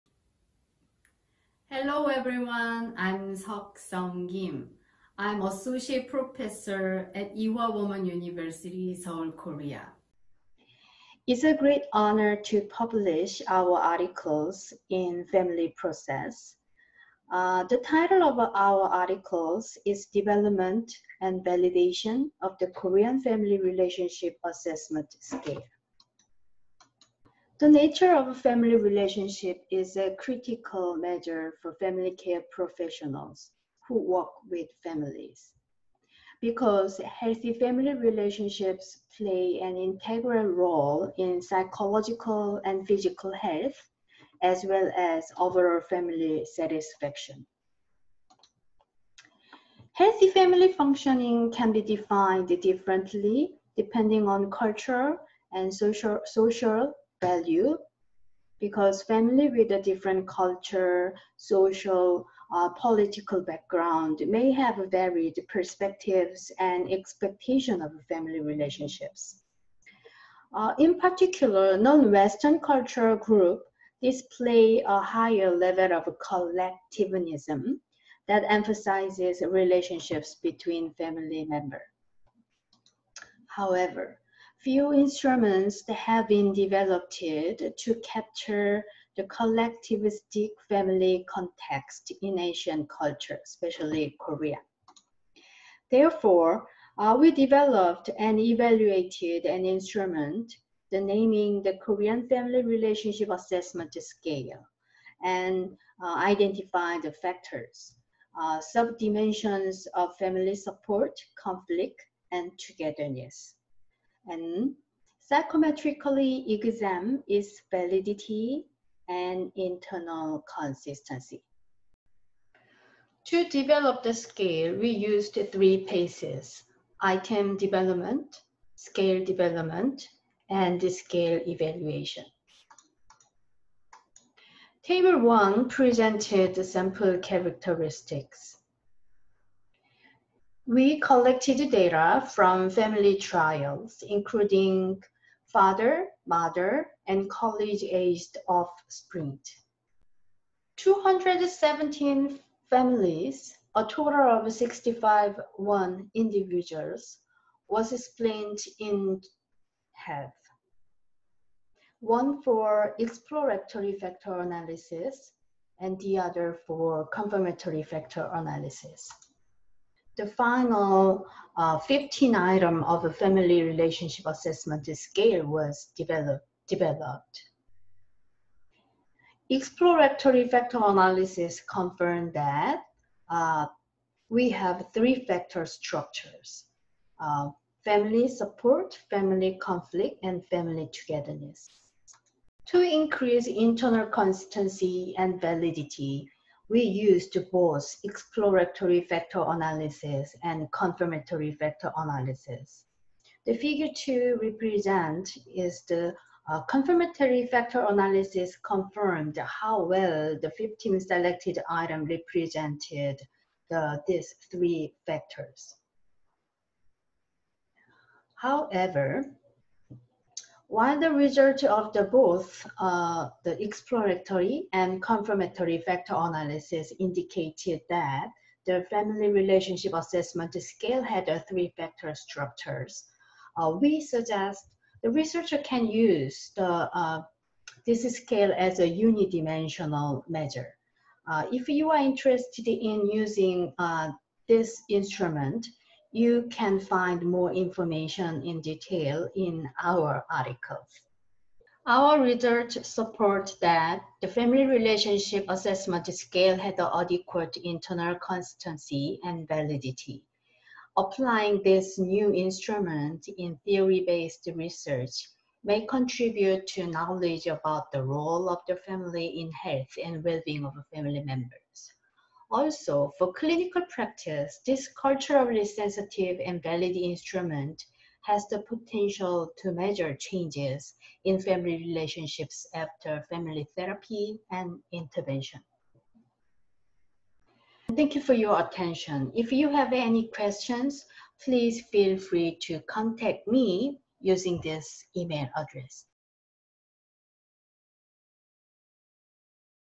The family relationship assessment scale
This paper reports the development and psychometric testing of a new scale, the Korean Family Relationship Assessment Scale (FRAS) with 15 items. The final FRAS demonstrated good internal consistency and validity with three factors: family support, family conflict, and family togetherness. It would be able to facilitate research on the relationships between family support, conflict, togetherness, and health, and may contribute to knowledge about the role of the family in the health and well-being of family members within a Korean context. For clinical practice, this culturally sensitive and valid instrument has the potential to measure changes in family relationships after family therapy or interventions.

Authors: Suk-Sun Kim, Minji Gil, Yeounsoo Kim-Godwin